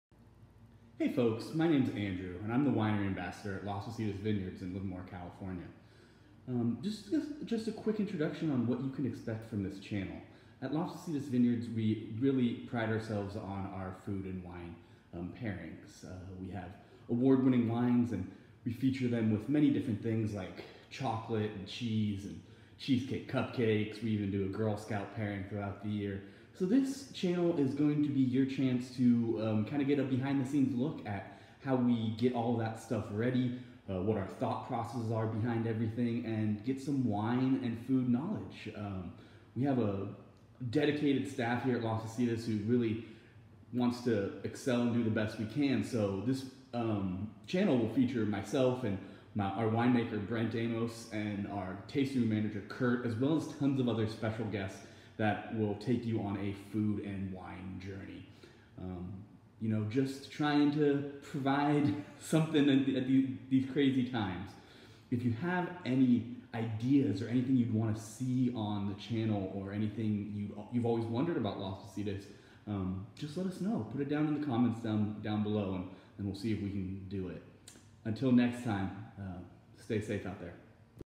Introduction to the Channel
Andrew gives an introduction to the LPV Channel.